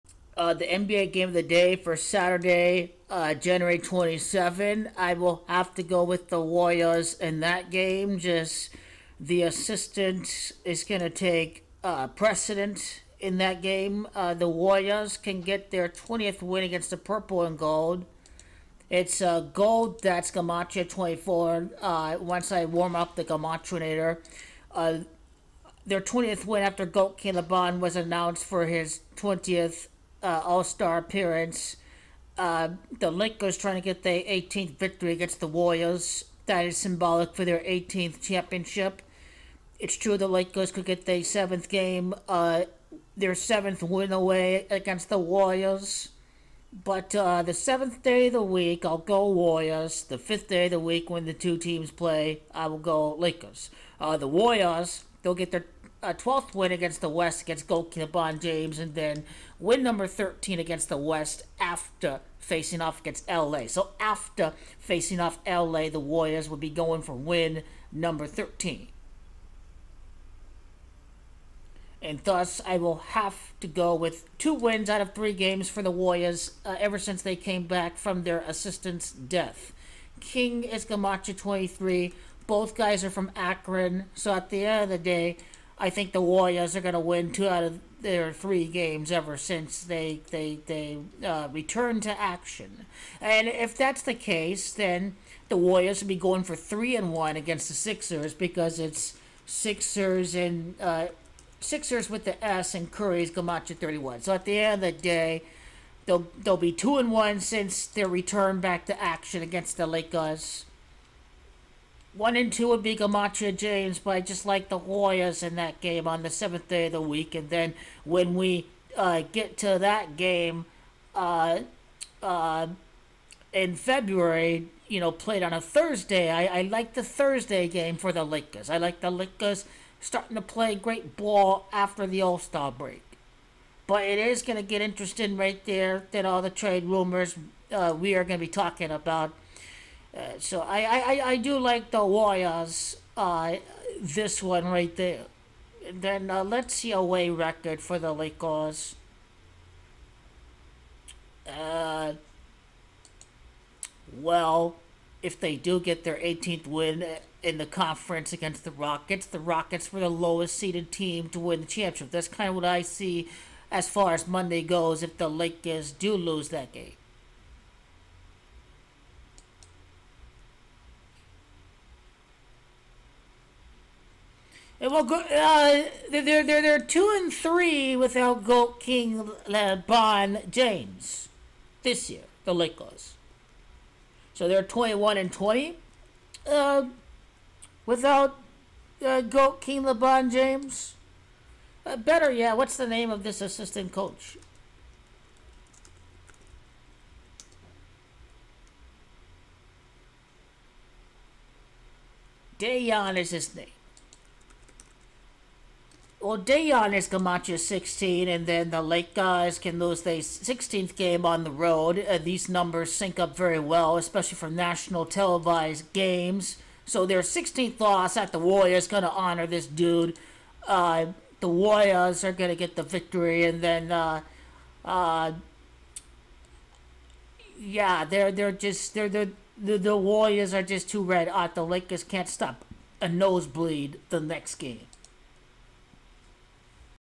2023-2024 NBA Game of Day January 27: Golden State Warriors-Curry Win vs LA Lakers-GOAT KING LEBRON!
LA picks up their 16th road loss of the season, as the Warriors win 2 out of 3 since returning to action following the death of their assistant Dejan. I like the 7x champs on the 7th day, and the next matchup on Thursday the 5th day of the week should favor the Lakeshow.